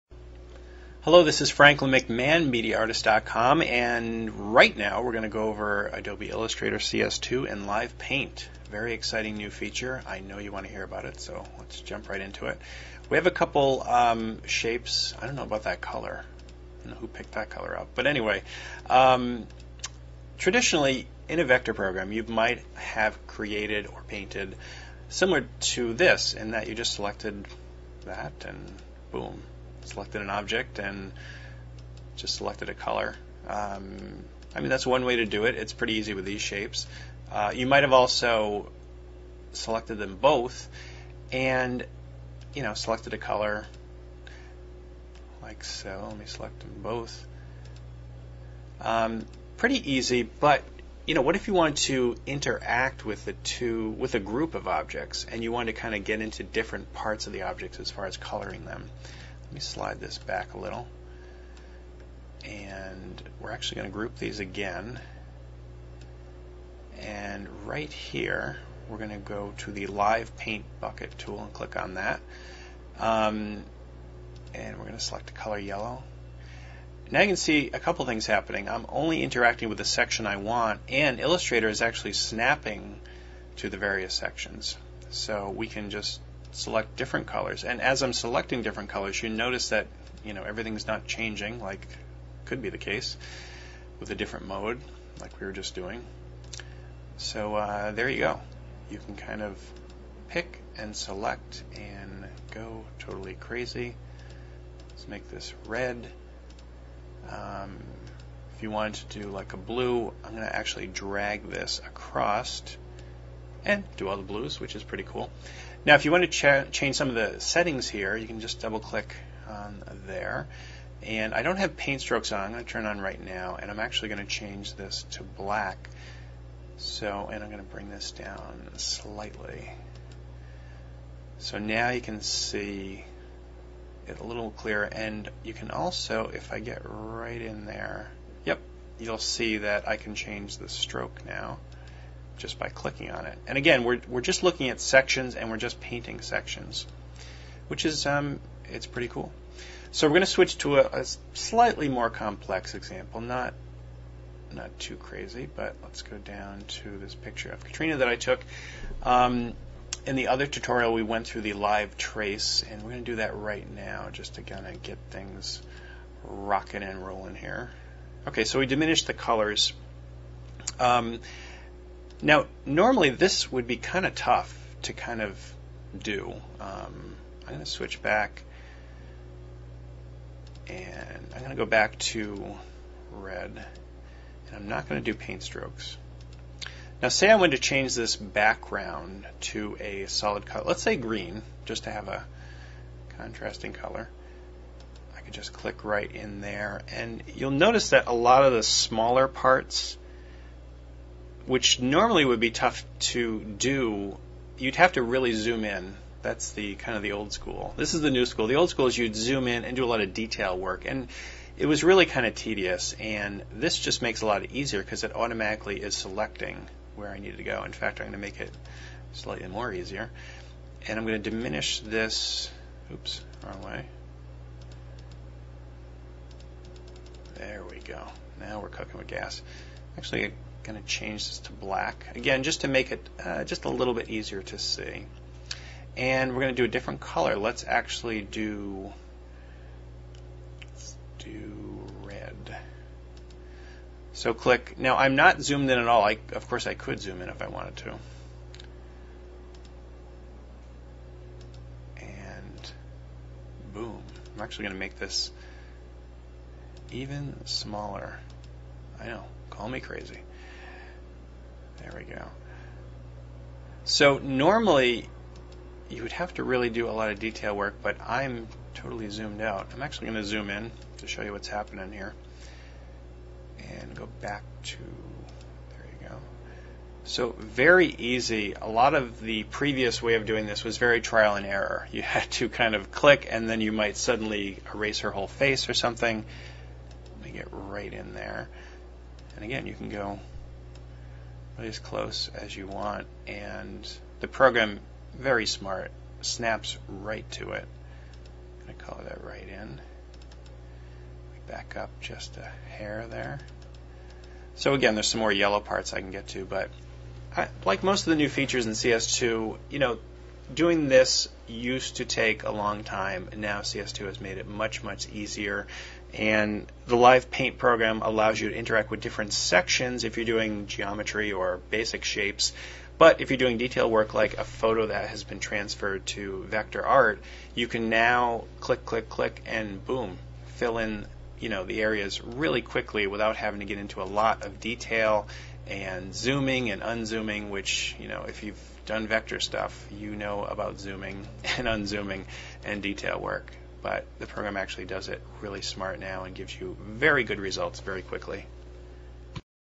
Inside Adobe Illustrator CS - Live Paint
In this video tutorial, Creative COW Leader Franklin McMahon demonstrates the new Live Paint feature in Adobe Illustrator CS 2 which makes coloring vector images a snap.